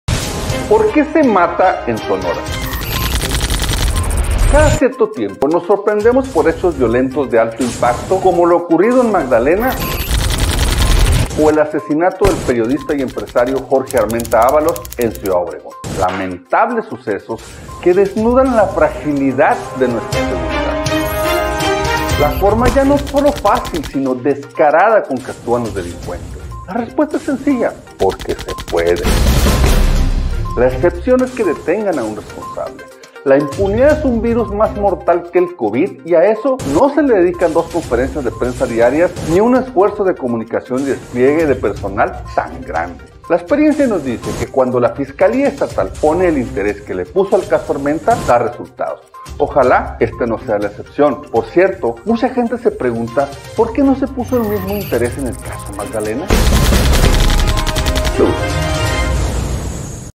MATAR EN SONORA
MATAR EN SONORA...Matar en Sonora parece convertirse en algo relativamente sencillo, alimentado por la impunidad. El que la hace no la paga. En 60Segundos abordamos el caso del periodista Jorge Armenta de Cajeme y la necesidad de castigar a los culpables. @60Segundos11